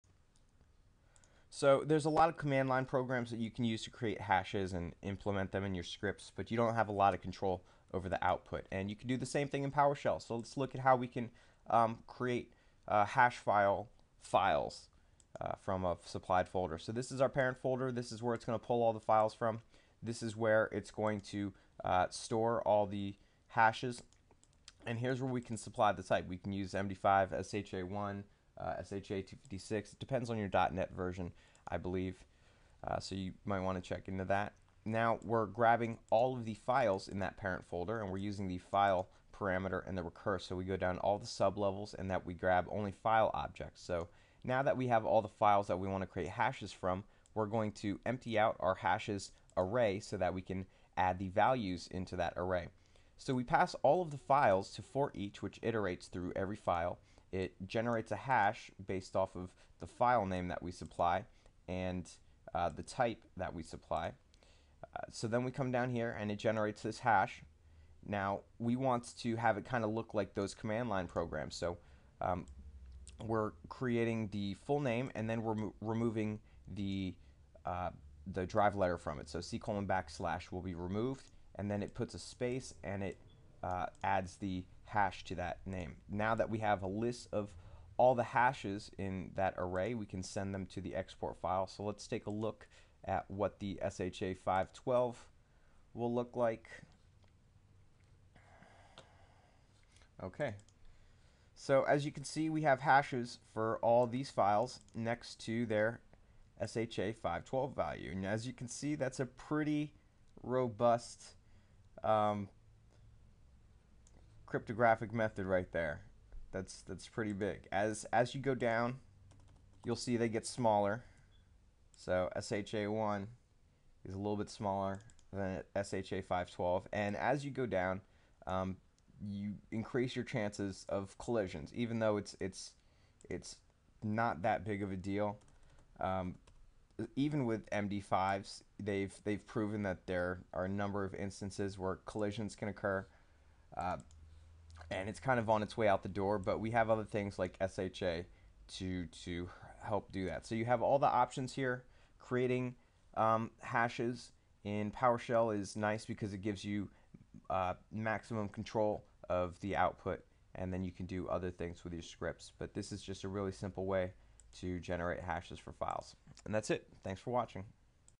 Create MD5/SHA1/More hashes from files in Powershell
Use Powershell and .NET to create hashes for files

I found this blog useful, and it contains a cmdlet version if you want to hash only one file